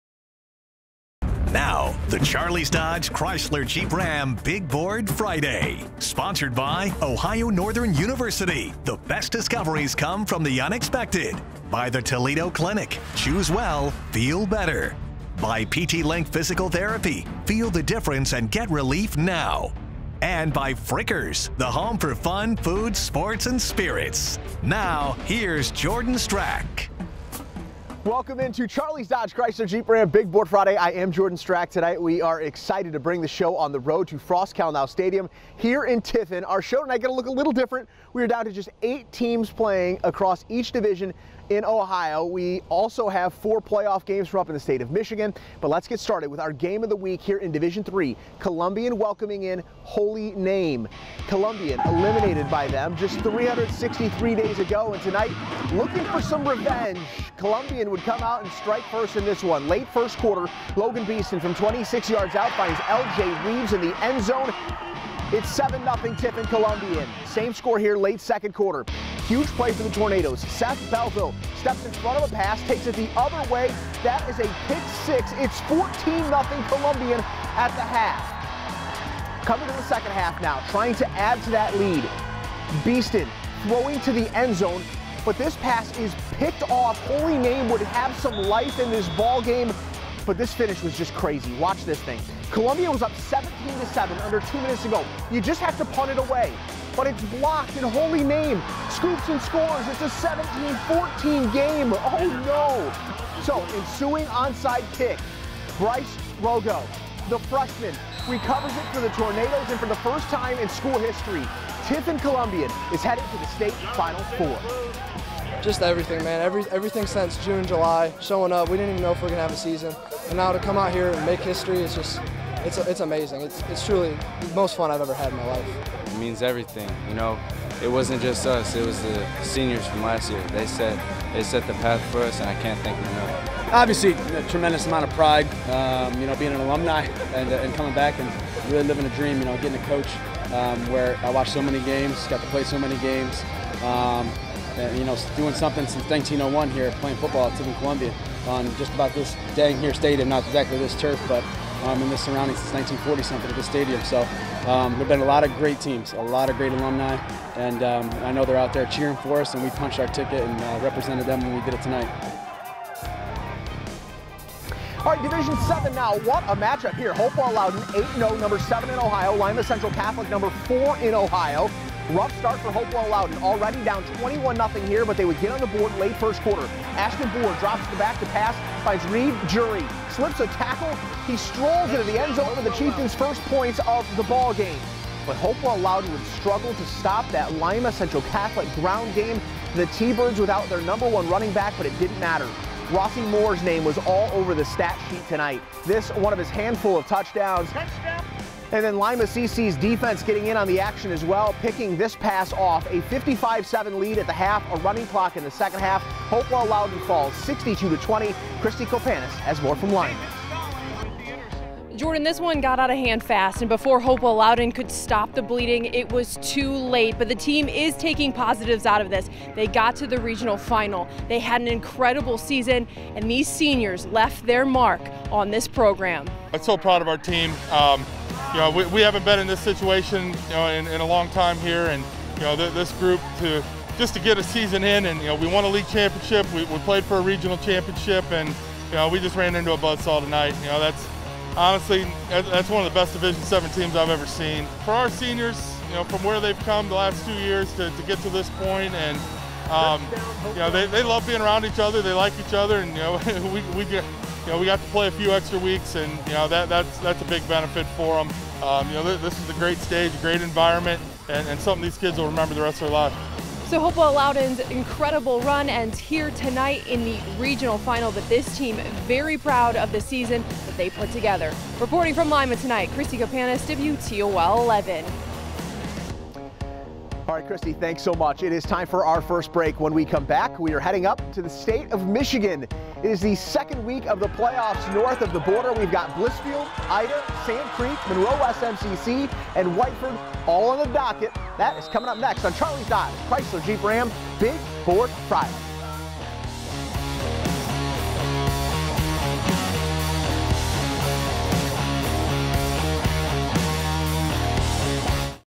Holy Name vs. Columbian and Hopewell-Loudon vs. Lima CC | Big Board Friday Week 11, Part 1
Here's the highlights from part 1:

Holy Name vs. Columbian
Hopewell-Loudon vs. Lima Central Catholic - 2:44